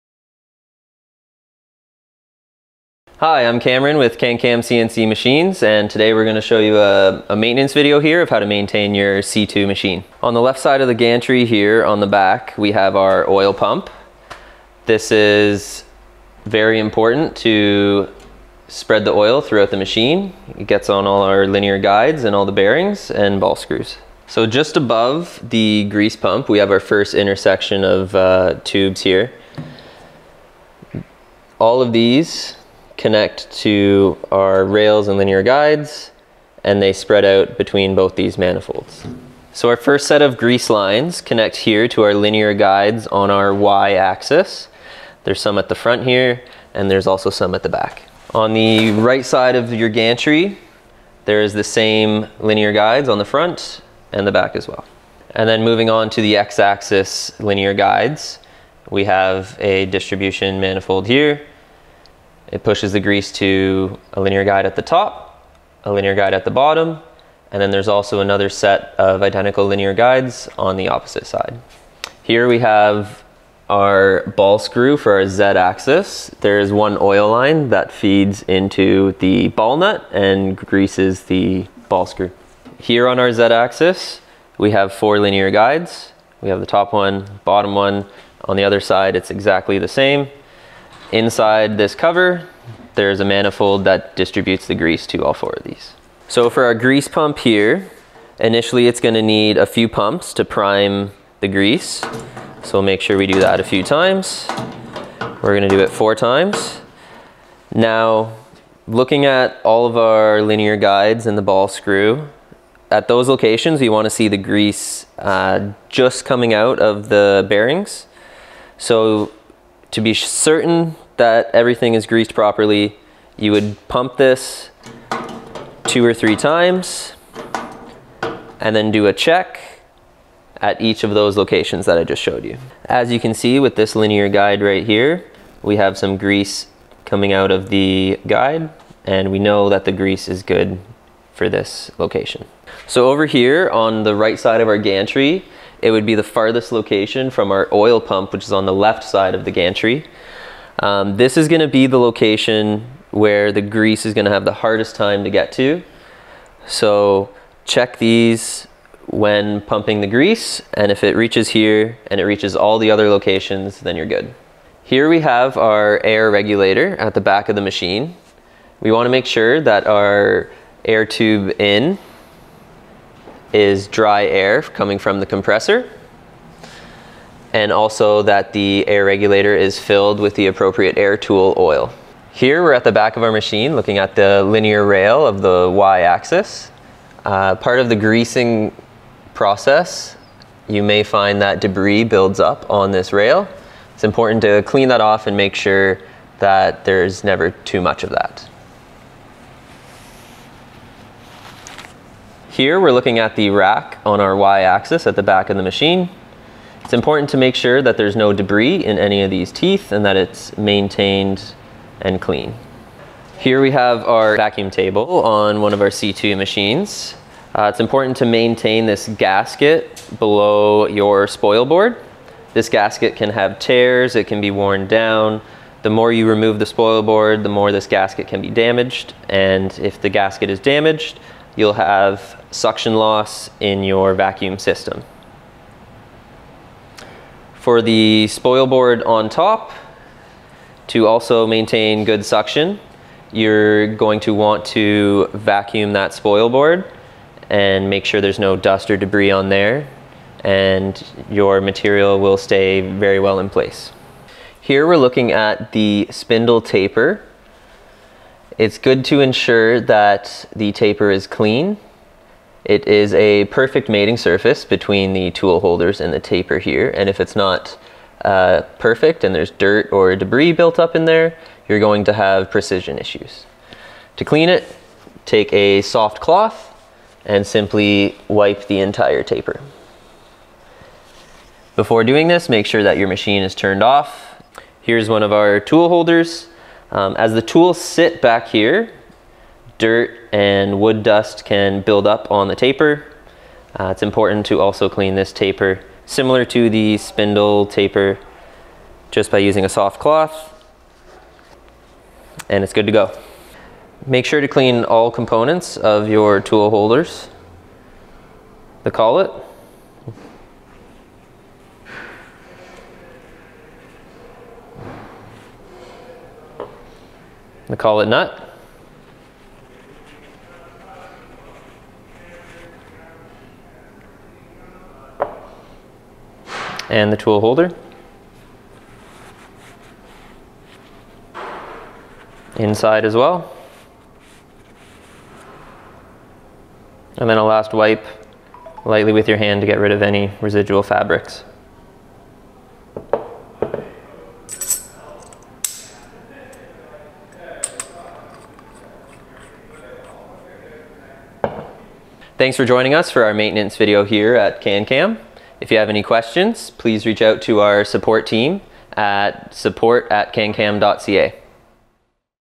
CanCam C2 series CNC router maintenance
In this video, Cameron runs you through the general maintenance of your CanCam C2 series CNC router.